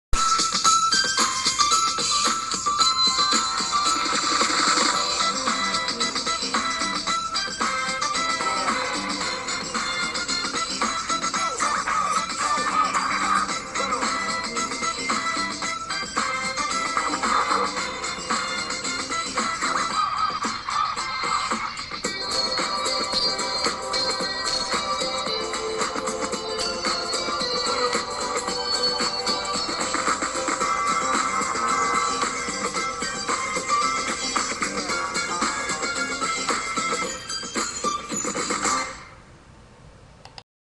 elfwosjig.mp4
WOS, decked out as an elf, does a hip hop jig.